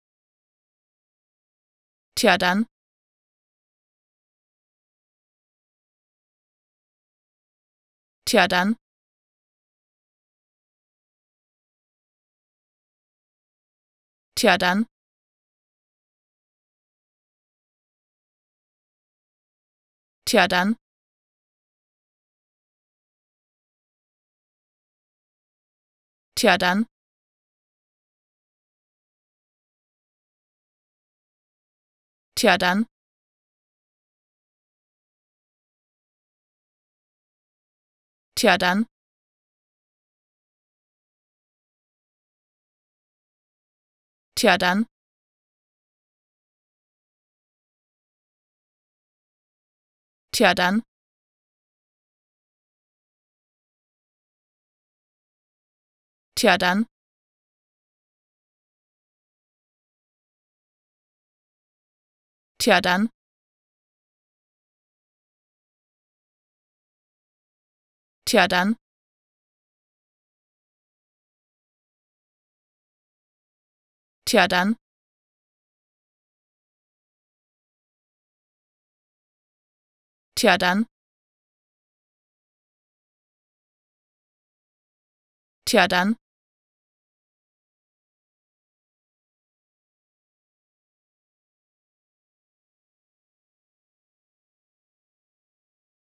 How to Pronounce ''tja dann'' (well then) Correctly in German
Learn how to say ''tja dann'' in German with this short pronunciation tutorial.